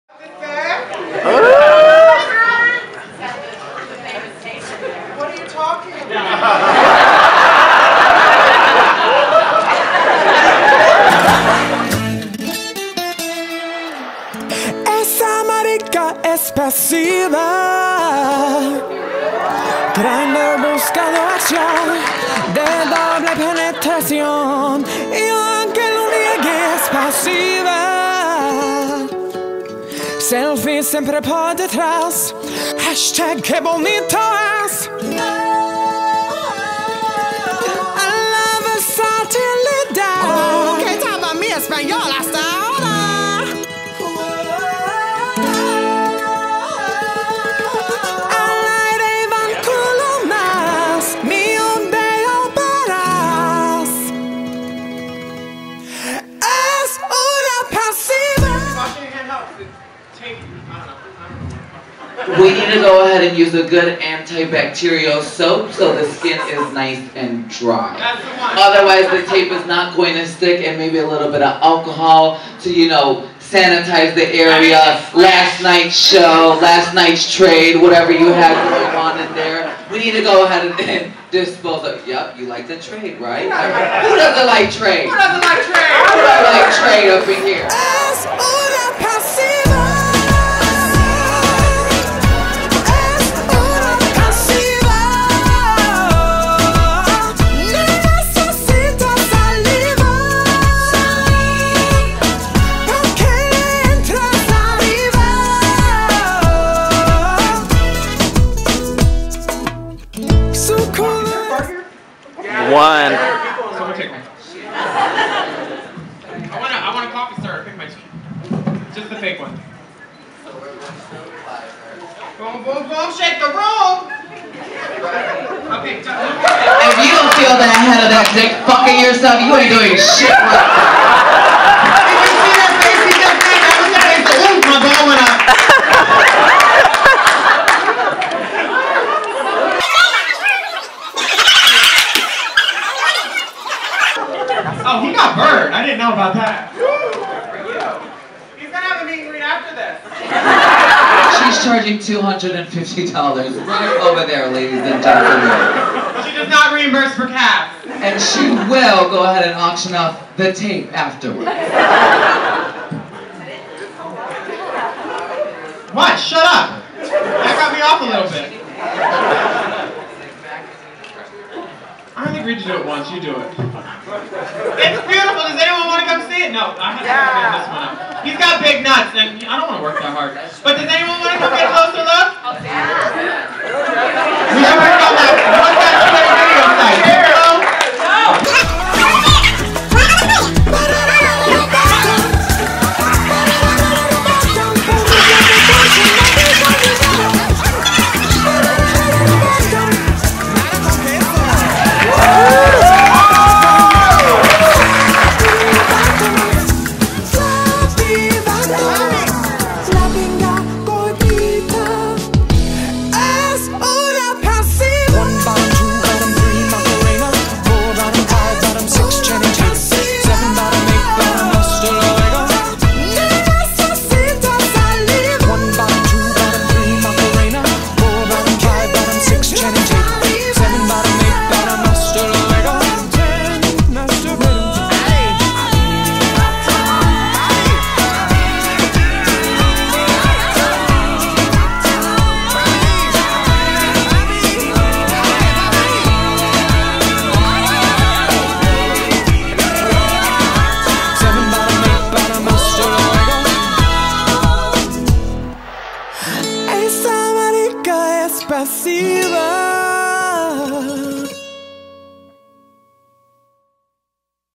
RuPaul's DragCon - Talking & Stalking (Willam Belli, Pearl, Trixie Mattel, Deven Green)
Talking & Stalking Part II

If you weren't there this is pretty much exactly how it was! We got some fun footage of the live tucking, meeting Willam & Deven Green! Pearl! Trixie!

Next Week: Interviews with Jujubee, Chi Chi Larue, TS Madison, RuPaul's Keynote & Latrice, & even more!

Now how many queens can you spot in this one video??

RuPaul's Drag Race is owned and produced by World of Wonder